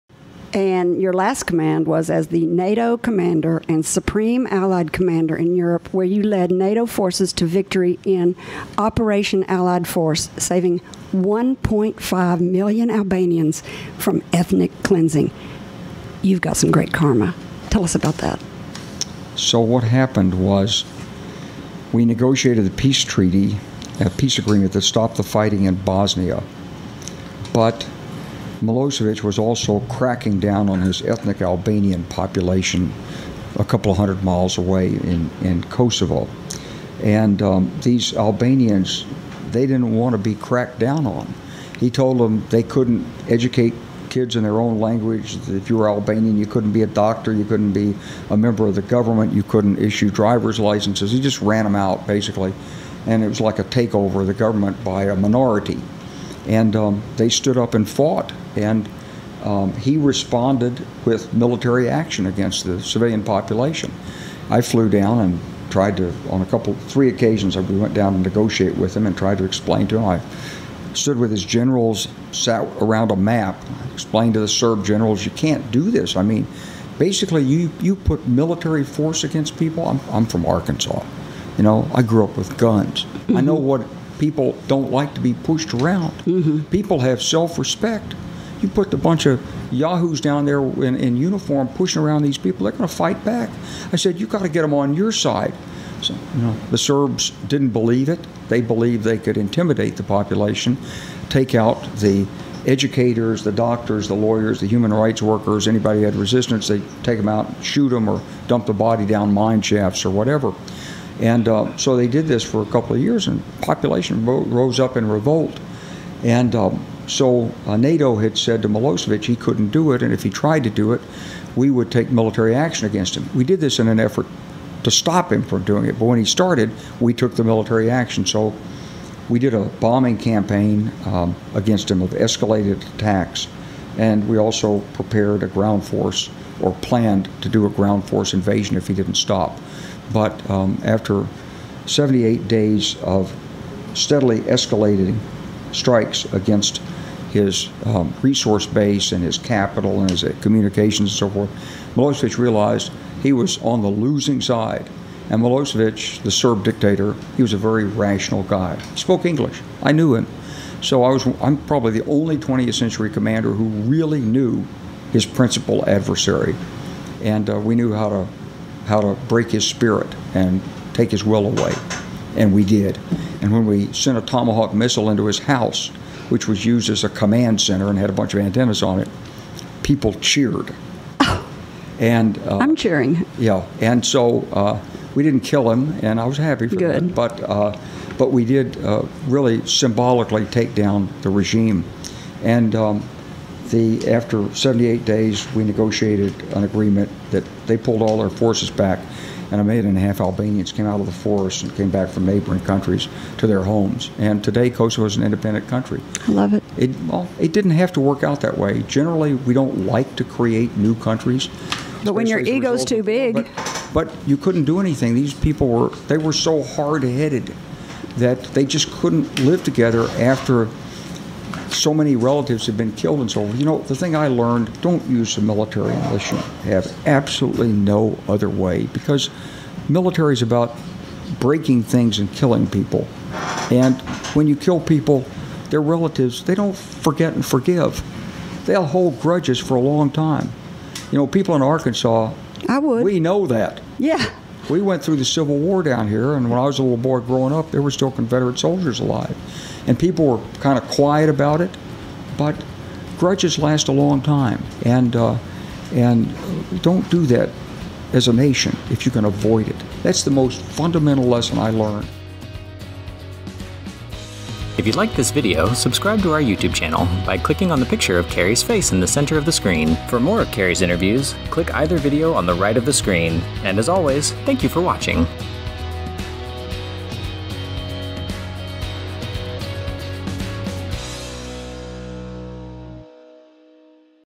How He Saved 1.5 Million Lives | The Highlights with General Wesley Clark
This week's guest is General Wesley Clark.

Never Miss a live show!